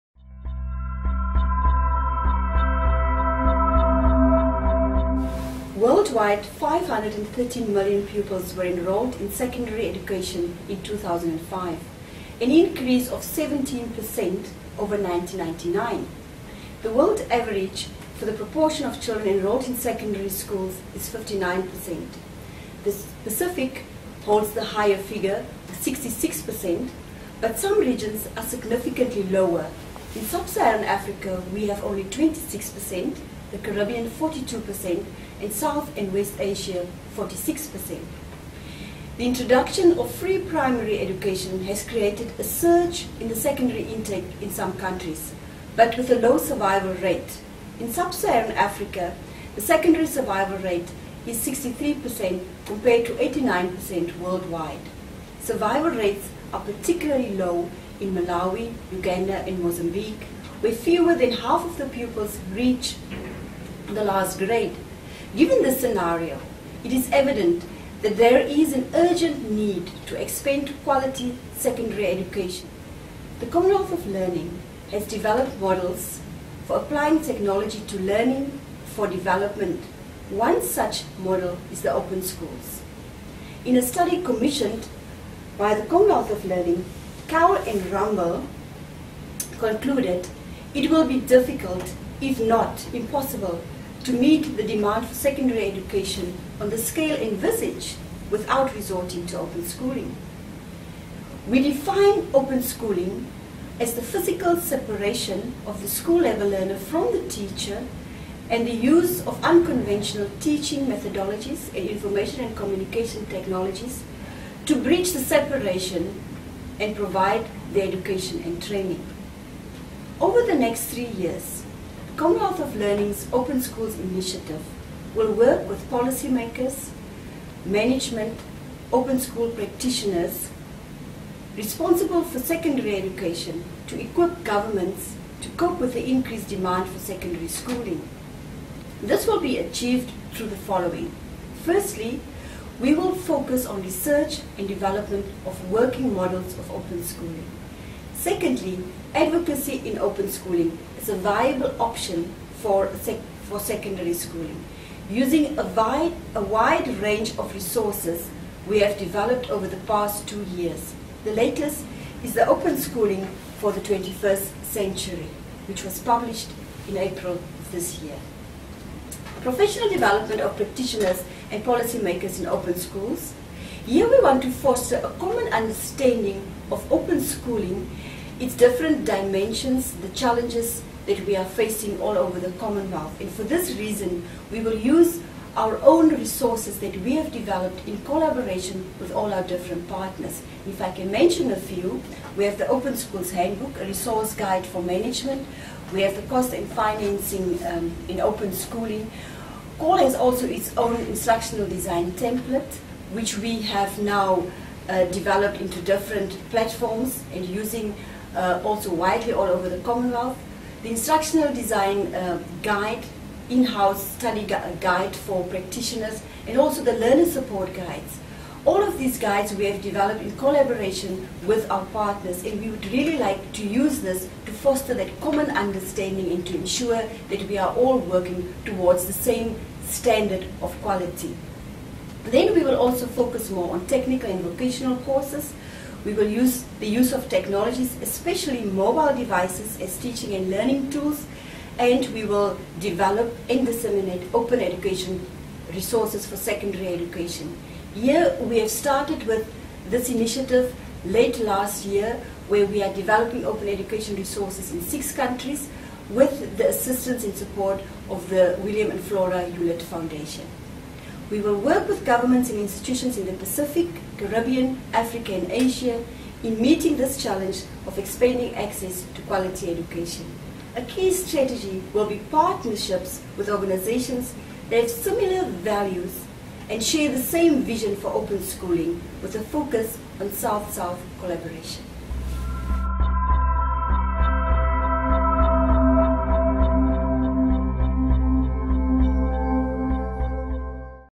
COL's Open Schooling Initiative
COL Education Specialist, Mrs. Frances J. Ferreira, presents COL's Open Schooling initiative for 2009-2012 in this five-minute video (2009)